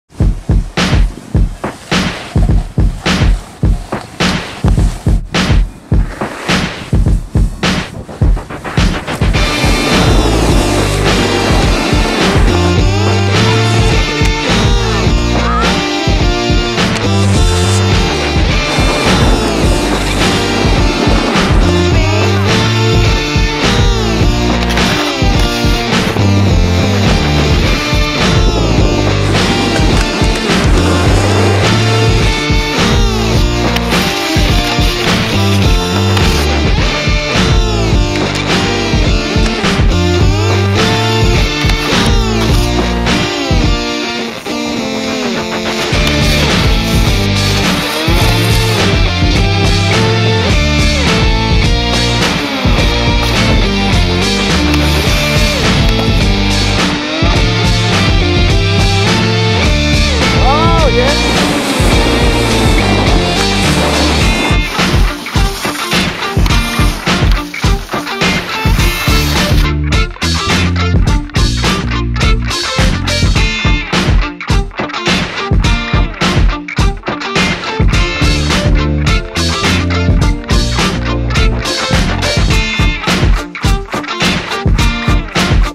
Comic Book of SnowPak
this is just a snowpak video with a comic book theme to it and it gives u a heads up on next years full length vid that we want to film all year for and it is called Snowpak "steppin it up" and it should be sick look for teasers and stuff of the middle of next year